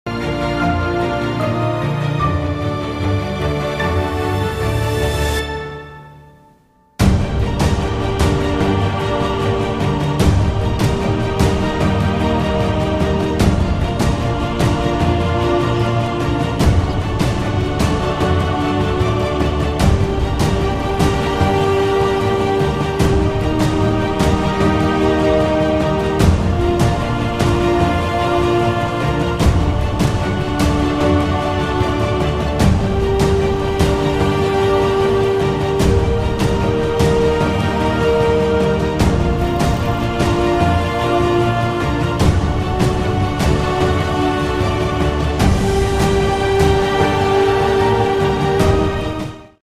Facts Roanoke colony dissappear mystery. #
Roanoke Colony was founded by governor Ralph Lane in 1585 on Roanoke Island in what is now Dare County, North Carolina, United States.Lane's colony was troubled by a lack of supplies and poor relations with the local Native Americans. While awaiting a delayed resupply mission by Sir Richard Grenville, Lane abandoned the colony and returned to England with Sir Francis Drake in 1586. Grenville arrived two weeks later and also returned home, leaving behind a small detachment to protect Raleigh's claim.[